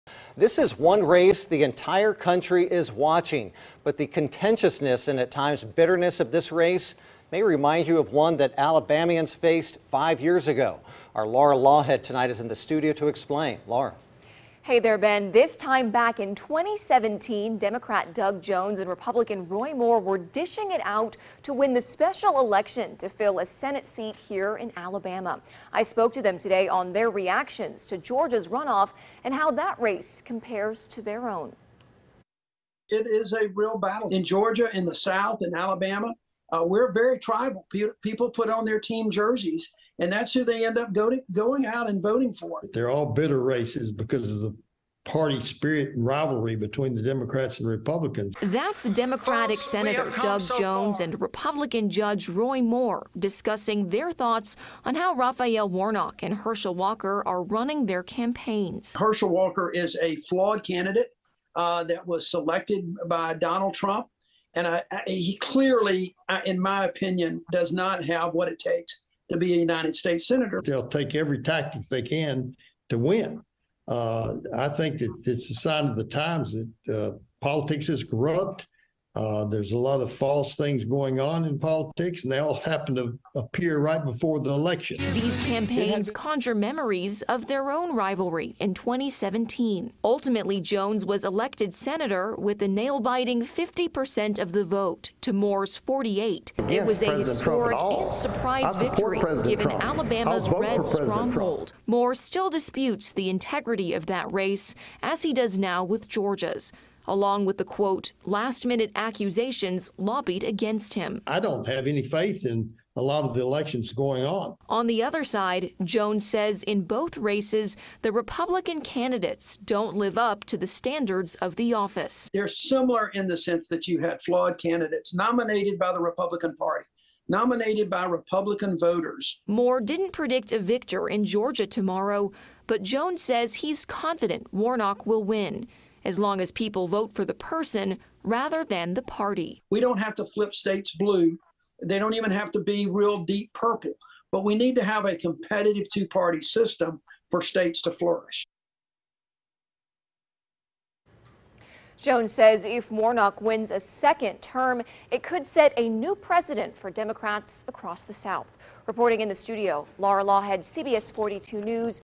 How Georgia Senate runoff compares to Alabama's 2017 Senate race
CBS42's Laura Laughead reports.